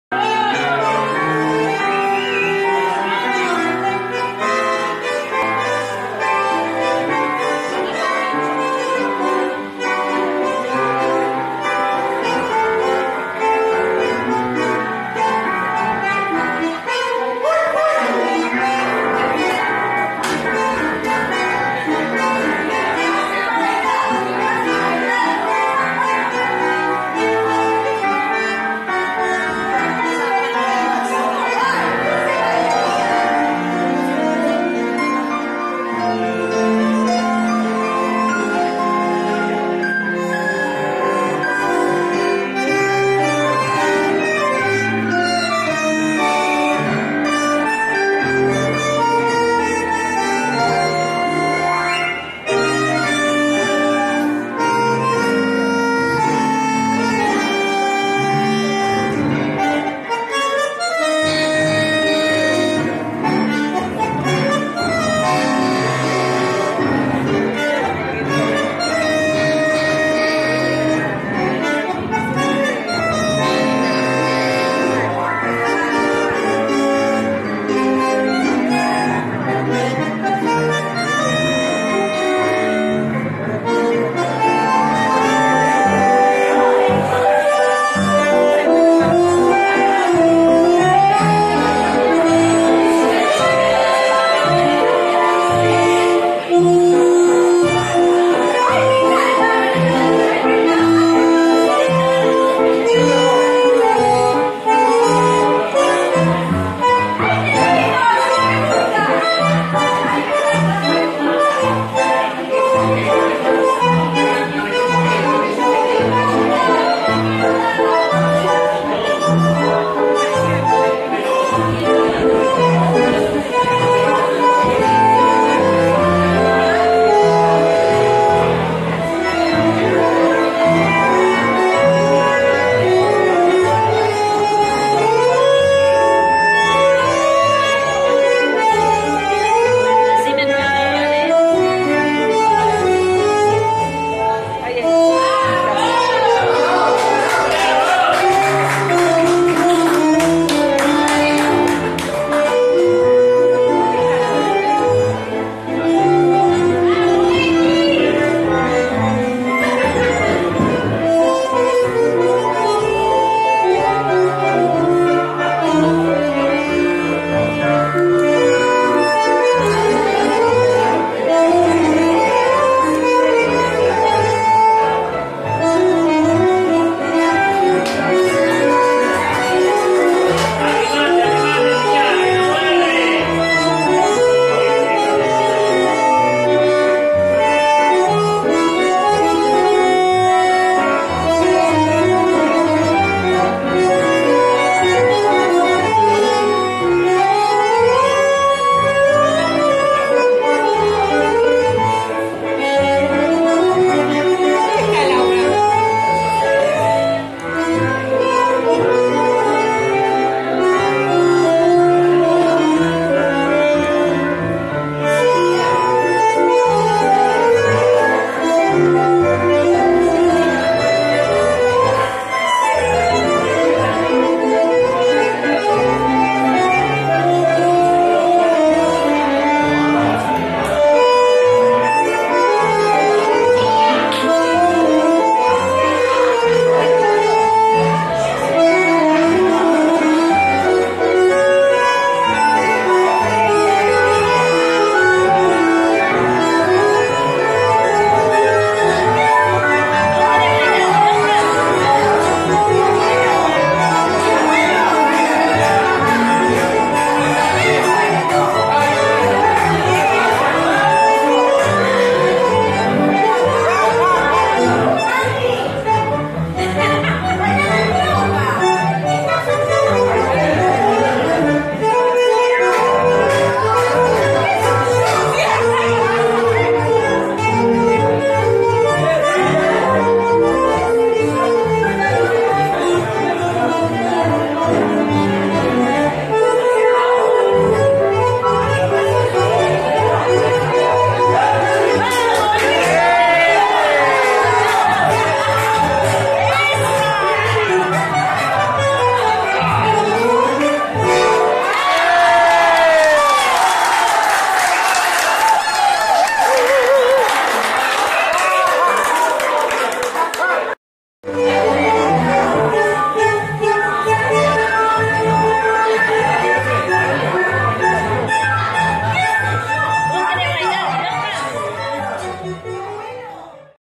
bandoneon solo en cumple de 80
video de cumpleaños de 80 privado
bandoneon solo
familia disfruta y baila con la homenajeada